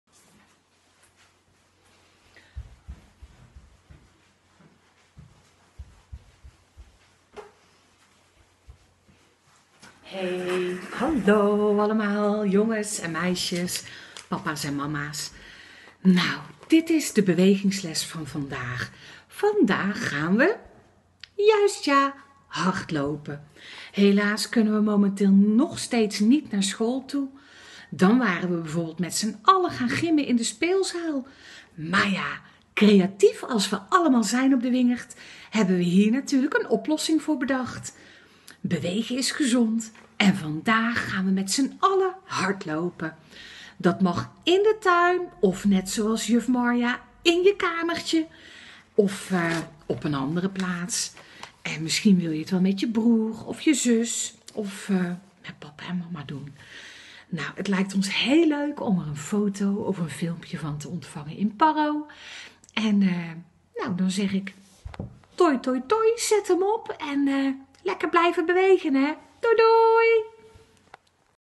Bewegingsles hardlopen
Juf Marja vindt het zo jammer dat we niet samen met jullie naar onze speelzaal kunnen of lekker rennen op ons mooie schoolplein. Maar ze heeft een plannetje: luister maar naar haar filmpje!